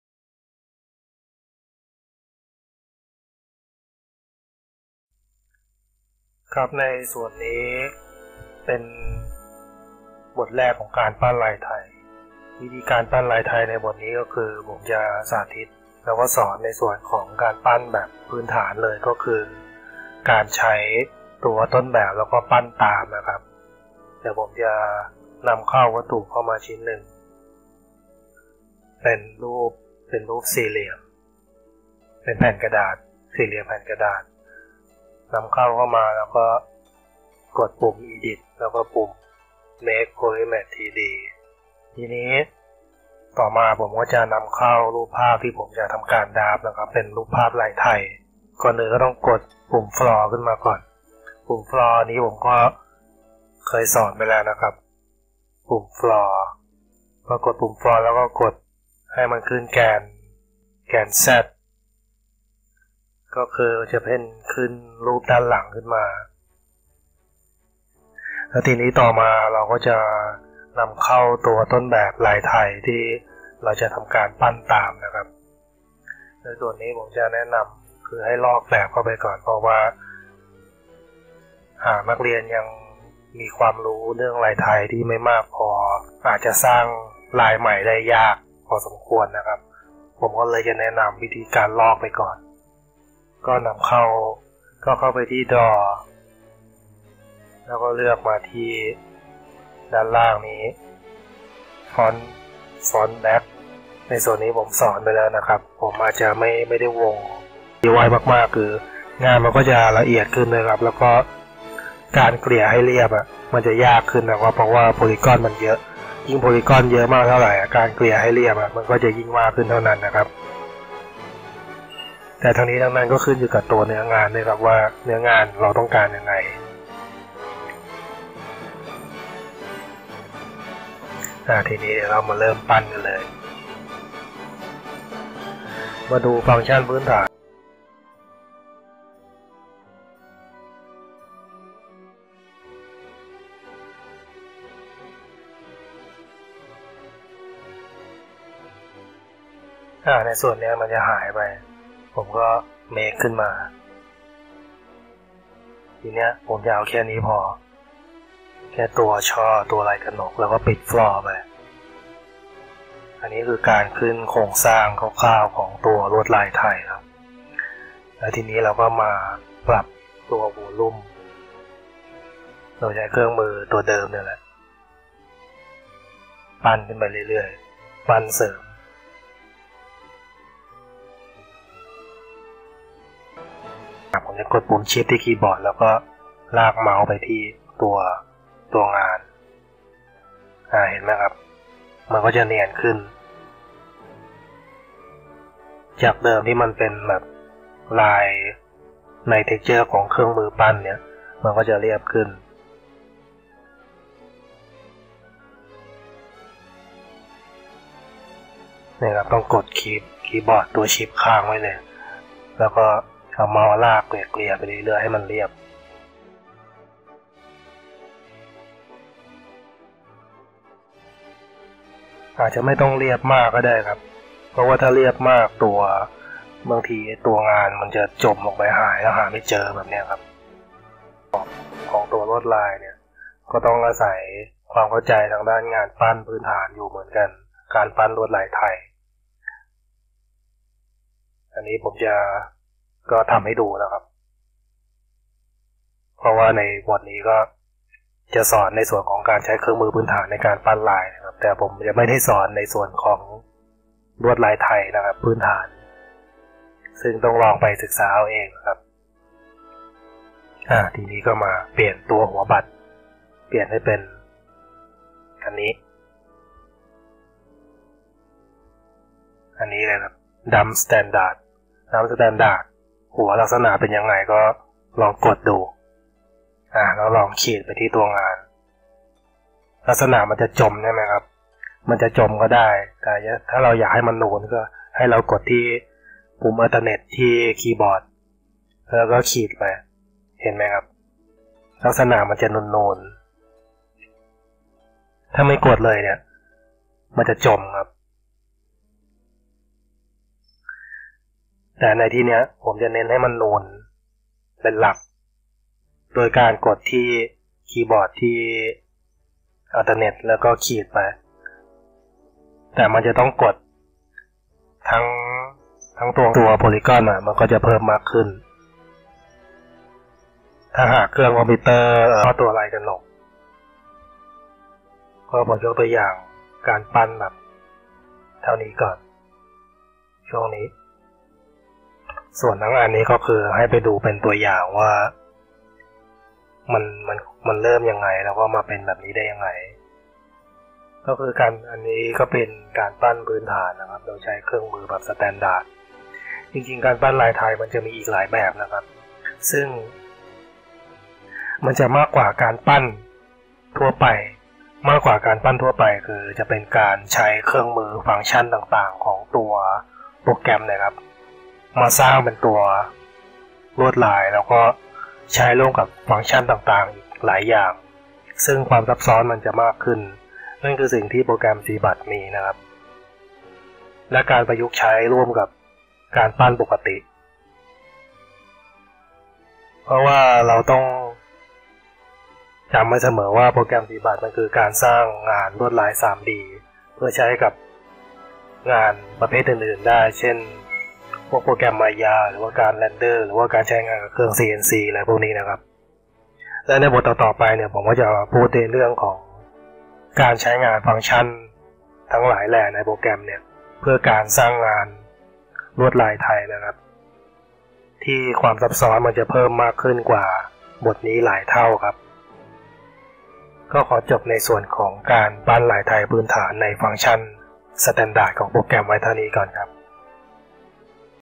หลักสูตร ออกแบ3D Zbrush งานพุทธศิลป์ - 26 ปั้นลายไทยด้วยเครื่องมือพื้นฐาน ดราฟตามแบบ
หลักสูตร การออกแบบ 3D สำหรับงานลายไทย  ด้วยโปรแกรม Zbrush เบื้องต้น-ระดับกลาง  (Update หลักสูตรปรับปรุง 2023) เรียนออนไลน์ ในกลุ่มเฟสบุค วิดีโอตัวเต็ม ดูได้ตลอดชีพ ไม่ลบ

หรือ
Inbox Facebook Fanpage : kuanporn Artist Thai